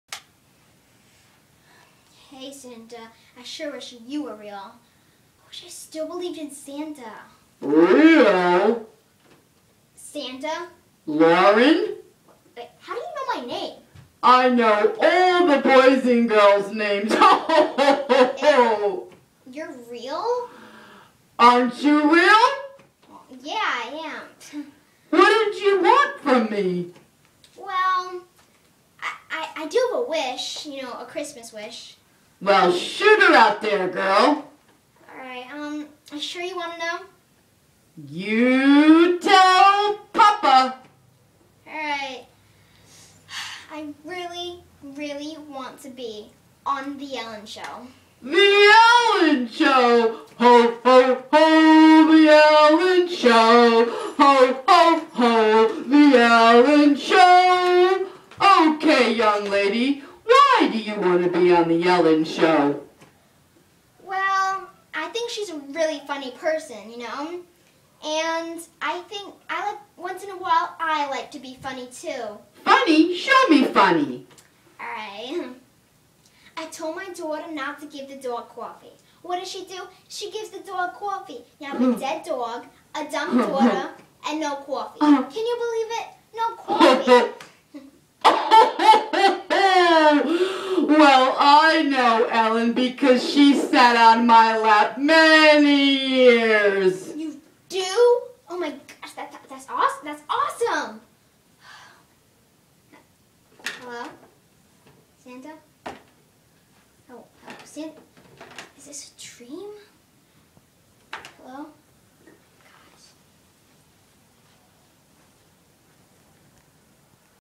Lauren Conklin's wish to be on the Ellen show!
Lauren Conklin, talking to Santa about wanting to meet ellen and be on the Ellen show!